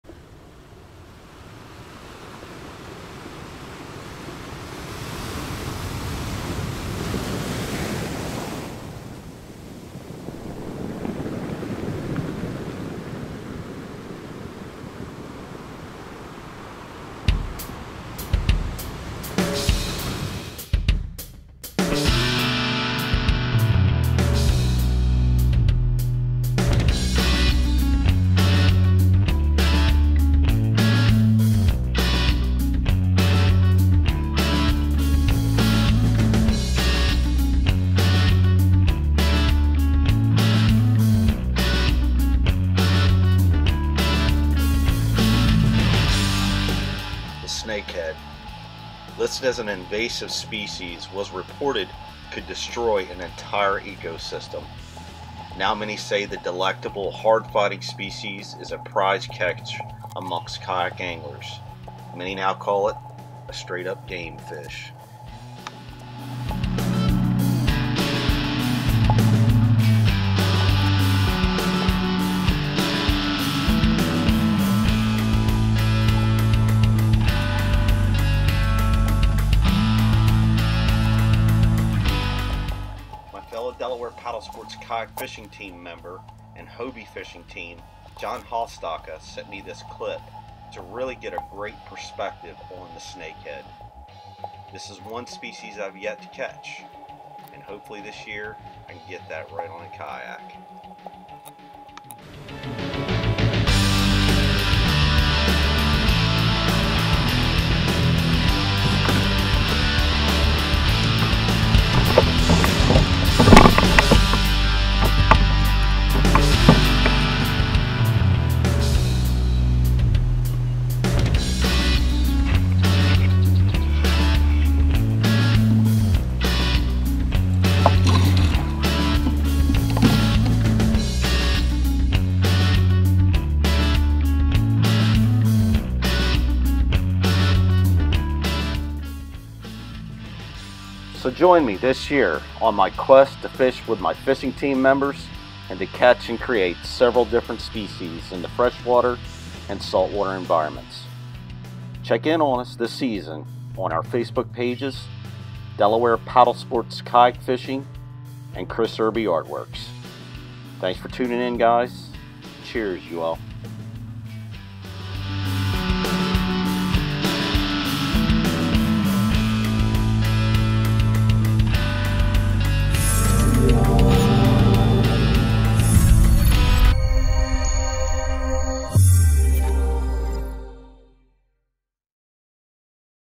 Delaware Paddlesports Snakehead
Chris Erby Artworks  creates the ever so popular Snakehead through acrylic paint. The species are invasive however, a many have chosen this as a epic catch from a kayak. Special thanks to my team member John Hostalka for the awesome clip. Visit us at delawarepaddlesports.com Facebook pages Delaware Paddlesports Kayak Fishing or Chris Erby Artworks